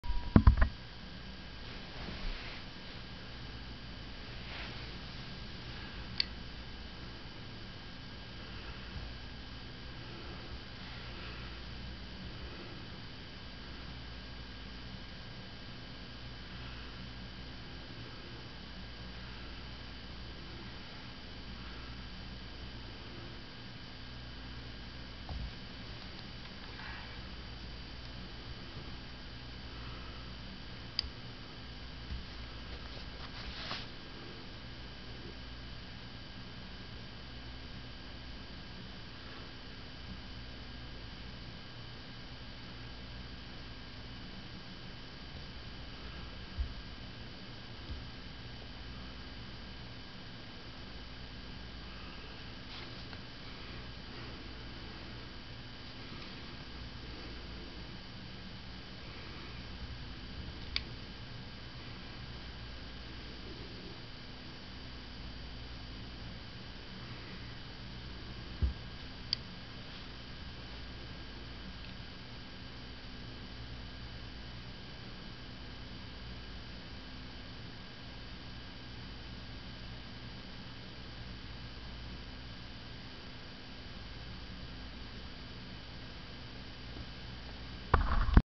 Lit Lichtenberg Figure Fixture
An LED stand for a Lichtenberg Figure lights up when it is picked up, glowing blue then white.  When the it is set down, it changes to yellow and fades.